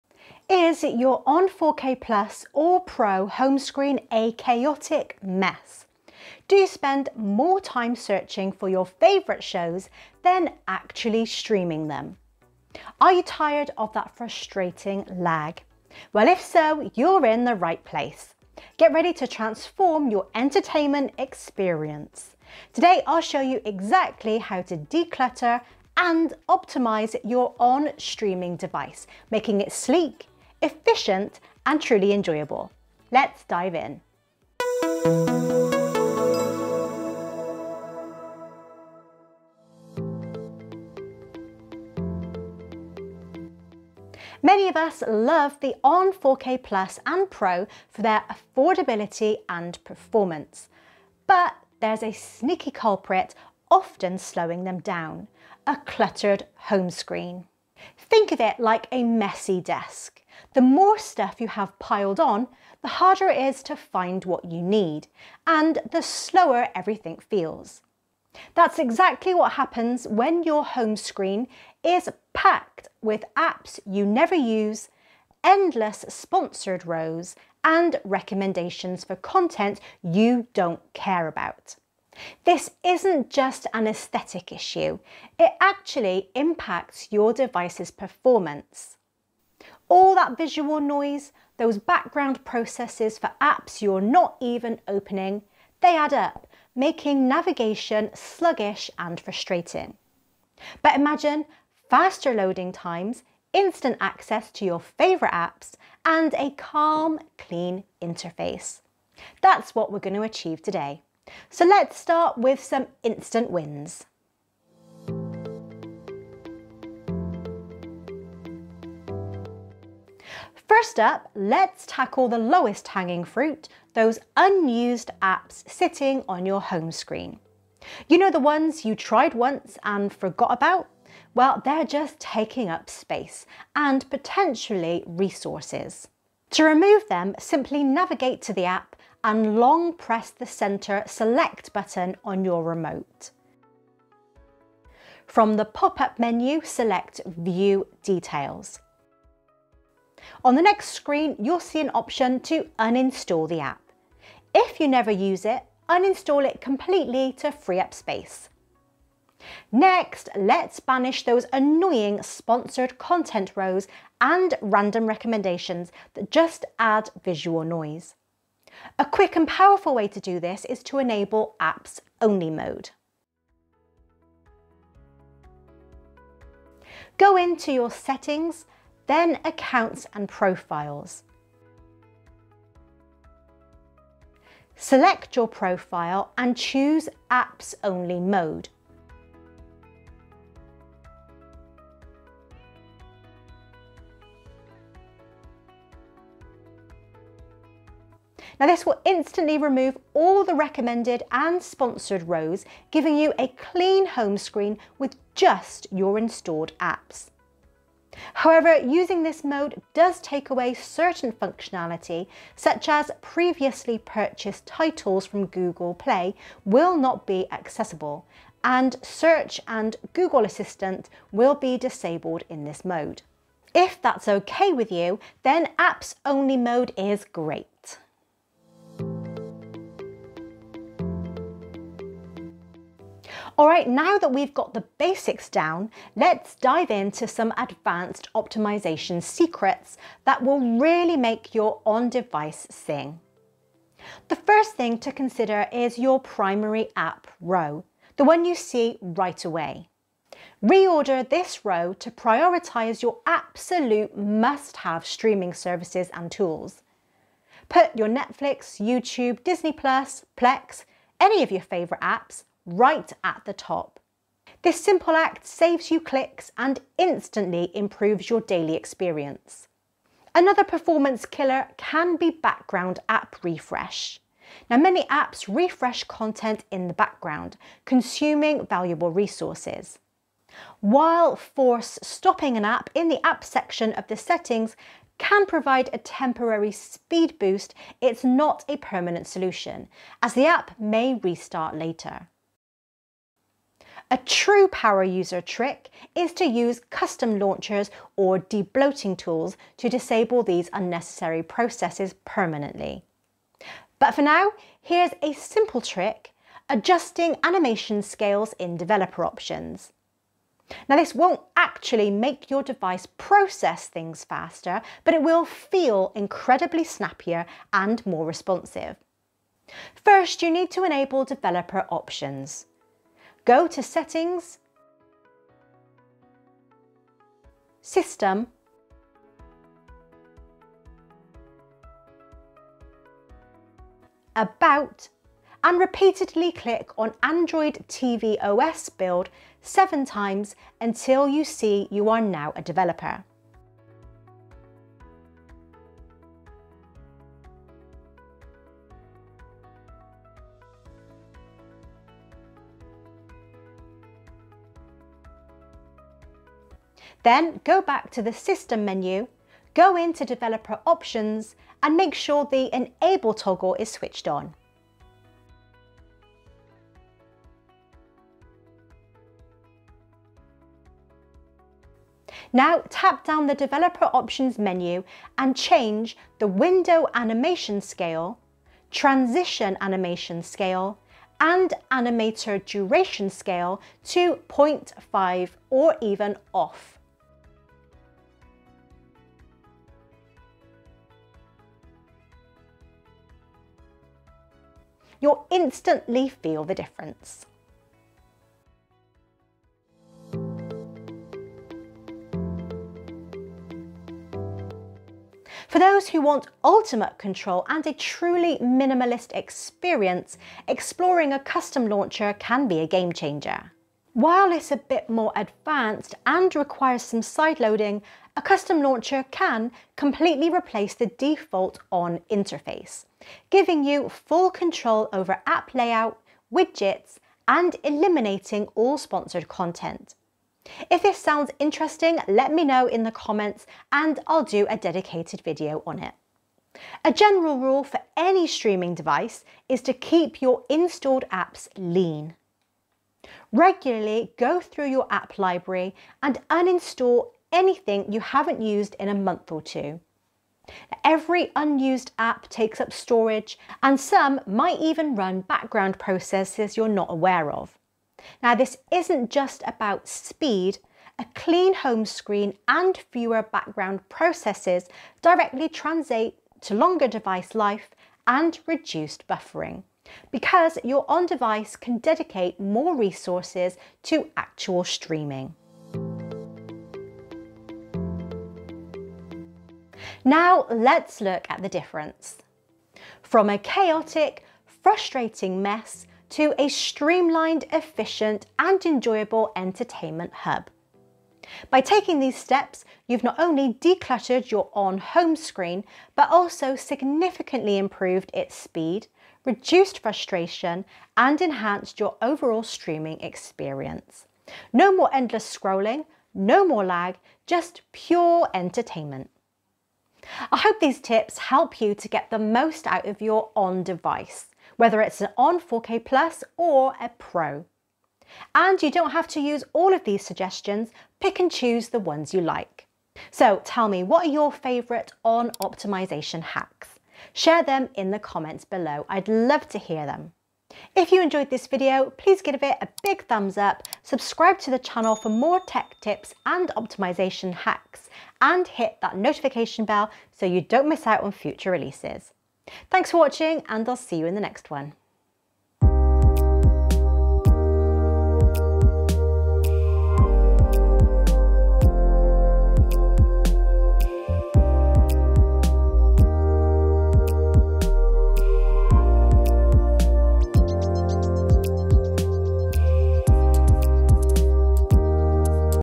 💡 Supercharge Your Onn 4K Plus & Pro: Banish Lag & Clutter! 🚀
Transform your Onn 4K Plus & Pro and get a clean home screen with faster performance! Is your streaming device slow and cluttered? This video will show you how to optimize your Onn to boost its speed and eliminate lag.

What You'll Learn:
✅ The Problem with Clutter: Understand why a messy home screen isn't just an eyesore—it actually slows down your device.
✅ Instant Decluttering: Learn how to quickly uninstall unused apps and enable "Apps Only Mode" for a clean, minimalist look.
✅ Pro-Level Optimization: Discover hidden tricks like reordering your app row and adjusting animation scales in Developer Options to make your device feel incredibly snappy.
✅ The Ultimate Fix: Find out how custom launchers can completely transform your Onn 4K device for maximum control and a pristine, ad-free experience.

Ready to reclaim your streaming experience? Watch now to get the most out of your Onn device!

🔗 Useful Links:
🛒 Onn 4K Remote Cover

⏱️TIMESTAMPS⏱️
0:00 - Intro
0:40 - Why Your Onn 4K Feels Slow
1:55 - Your First Steps to Decluttering
3:49 - Advanced Optimisation Secrets
6:42 - Maximising Your Onn 4K Experience
7:56 - A Streamlined Future
8:30 - Close & Outro